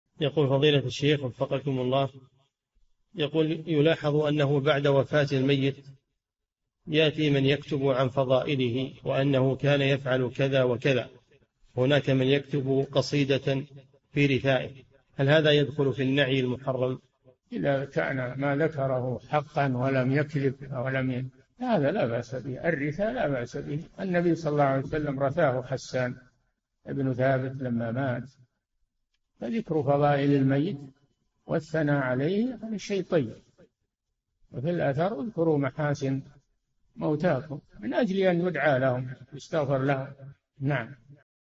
[4146- 4717] هل من النعي المحرم الكتابة عن فضائل الميت وكتابة الشعر في رثائه؟ - الشيخ صالح الفوزان
"[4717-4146] يلاحظ أنه بعد وفاة الميت يأتي من يكتب عن فضائله وأنه كان يفعل كذا وكذا وهناك من يكتب قصيدة في رثائه فهل هذا يدخل في النعي المحرم؟
الجَوَابُ: إذا كان ما ذكره حقًا ولم يكذب، فهذا لا بأس به، الرثاء لا بأس به.
 النبي ﷺ رثاه حسَّان بن ثابت لمّا مات فذكر فضائل الميت والثناء عليه هذا شيء طيب، وفي الأثر: «اذكروا محاسن موتاكم» من أجل أن يدعى لهم ويستغفر لهم.

للاشتراك في قناة الشيخ صالح الفوزان
فتاوى المجالس في تفسير المفصل

للاستفسار والإقتراحات والدعم
رئيس الفريق
لمشروع كبار العلماء